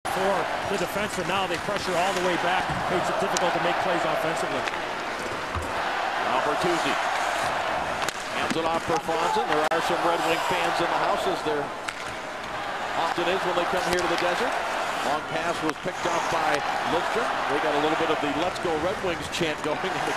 Lets go Red Wings Chant in Phoenix Feb 6 2012
Red Wings vs Coyotes